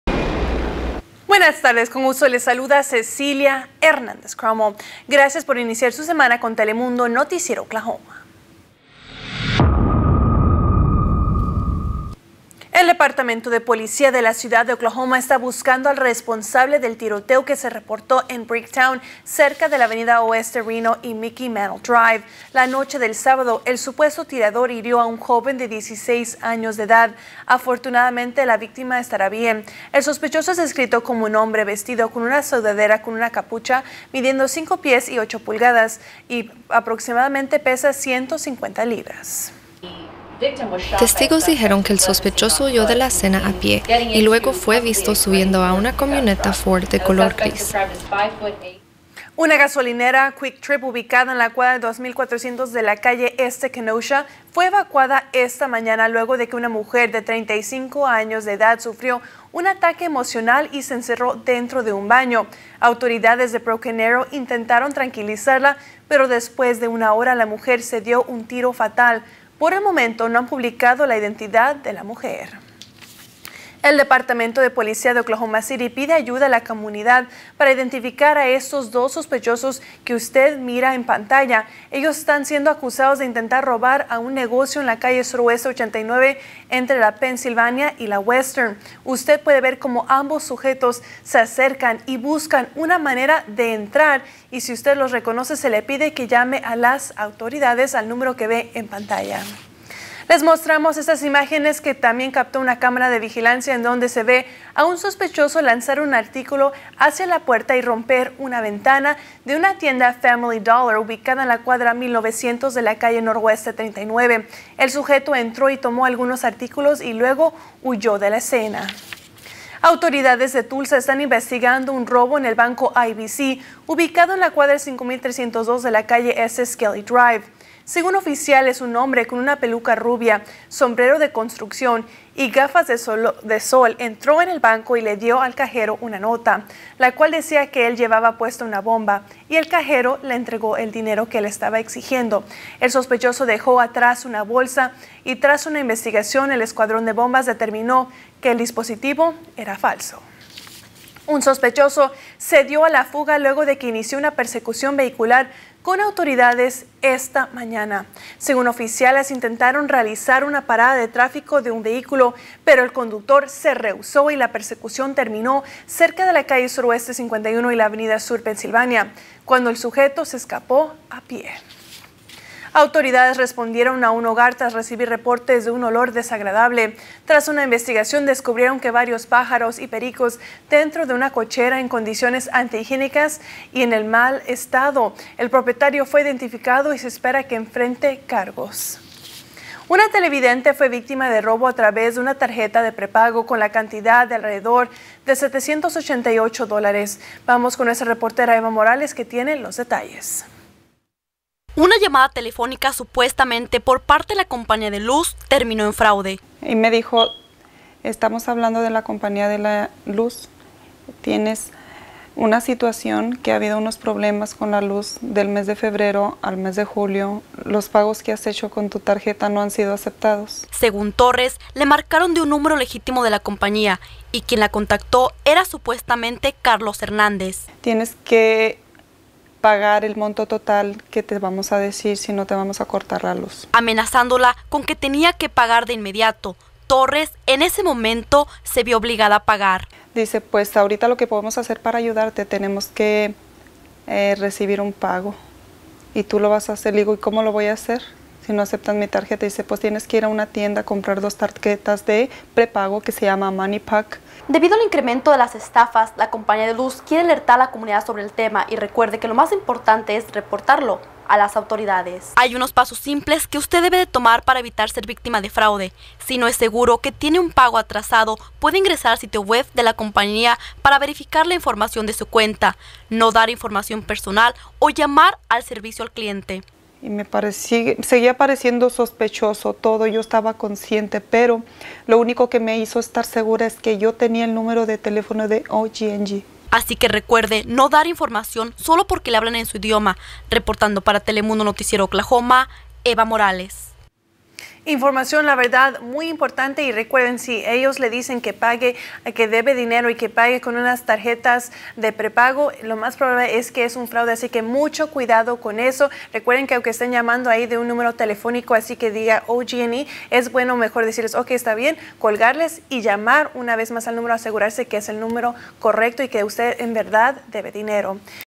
CRIMEN JULIO 1
Una mujer es víctima de estafas mientras que  en otra ciudad un atrincheramiento terminó con un homicidio.